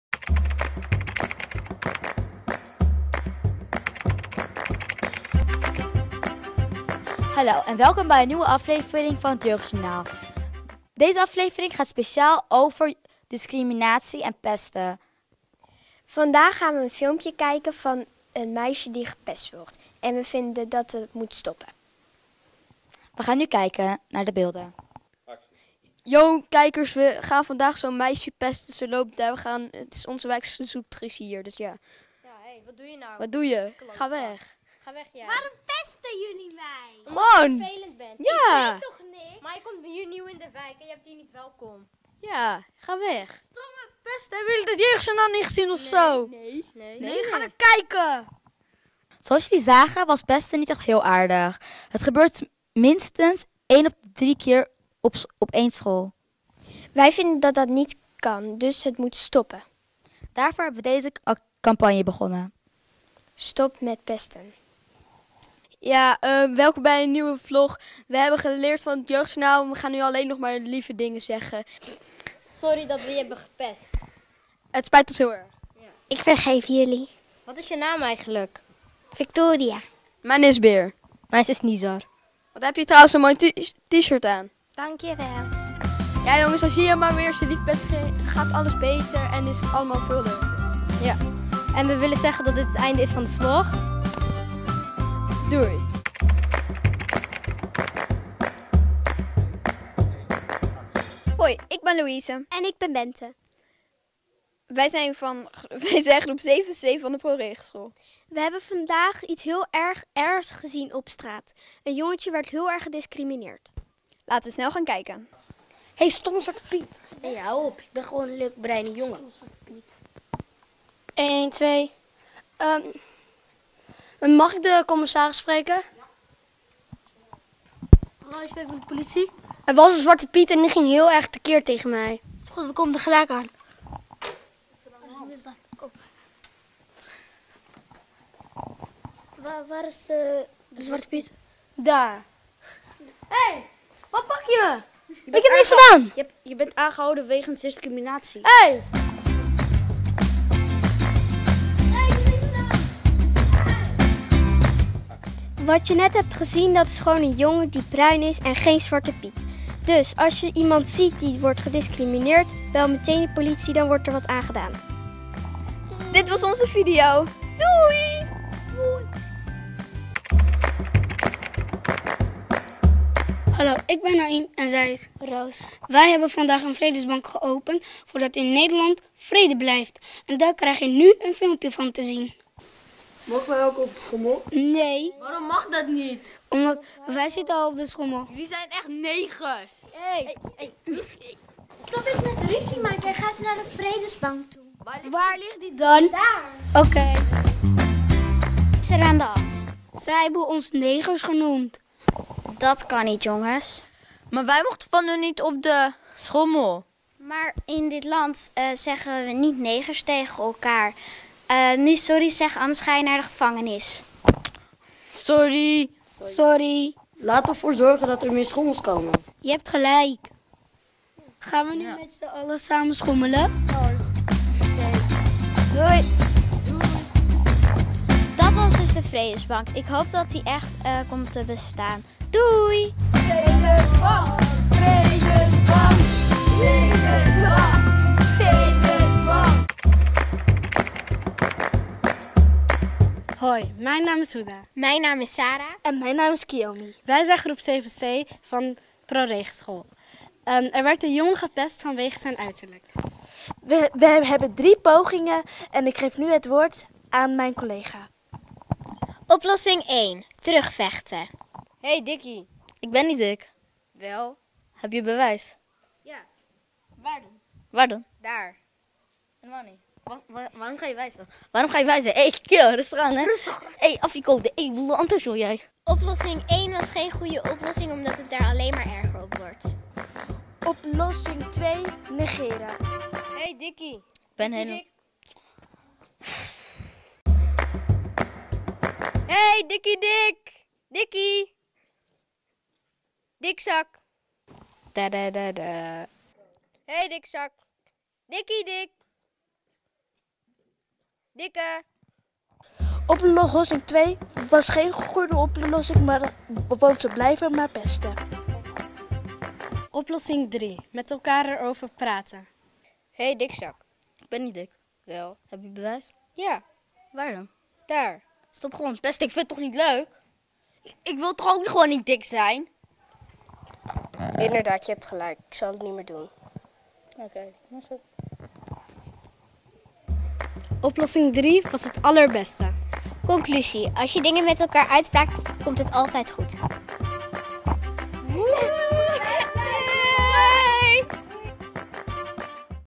Diversiteitsproject van Golypoon School 5 prorege 7c Amsterdam
In het Golypoon Diversiteitsproject zoeken leerlingen van groep 7oplossingen en antwoorden voor het thema 'Diversiteit'. Een hele dag gaan ze aan de slag met vooroordelen, gewoonten en vanzelfsprekendheden. Ze analyseren problemen & taboes rondom dit onderwerp en formuleren hiervoor antwoorden en oplossingen. Uiteindelijk verwerken ze deze tot een unieke film die wordt gepresenteerd aan ouders, familieleden en andere belangstellenden.

Het project loopt van 12 januari tot medio april. Het is de bedoeling om alle 30 films te overhandigen aan B&W van Amsterdam met de wens om een krachtige, creatieve en originele bijdrage te leveren aan het vraagstuk 'samenleven in Diversiteit'.